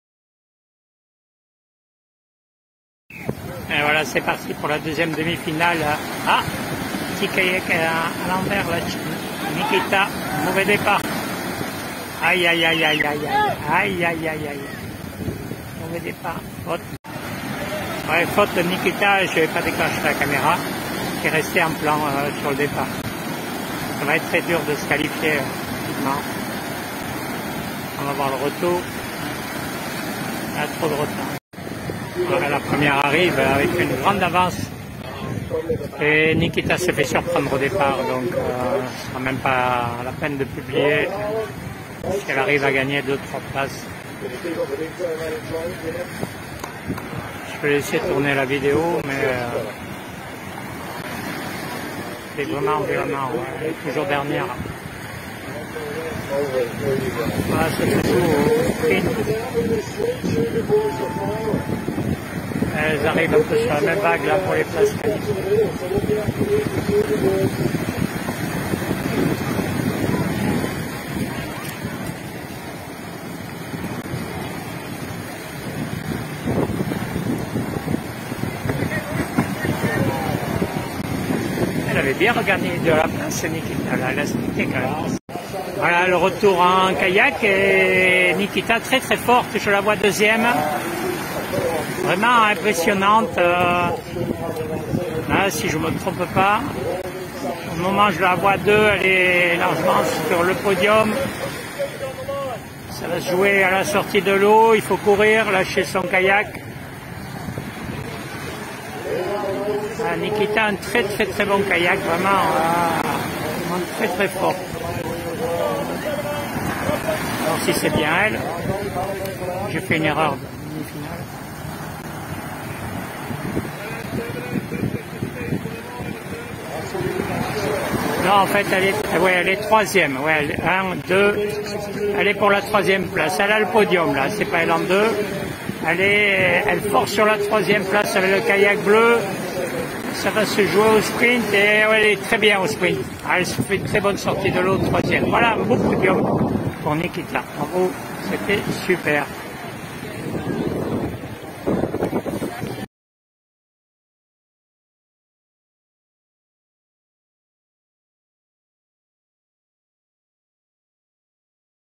Summer of Surf Maroochydore womens Surfski semi and final U17 - Never Ever Quit +subs
Cela commence avec une perte de tout espoir du vidéaste Popeye dû à  la chute au départ de Nikita  la kayakiste. Mais Nikita lui prépare une surprise !
Merci de ne pas avoir arrêté ta caméra Popeye, il faut toujours y croire jusqu'au bout !
Film et commentaire "by Popeye" de la demie et de la finale de la course surfski (kayak) des filles U17 (moins de 17 ans) du Summer of Surf de janvier 2023 à Maroochydore (Australie).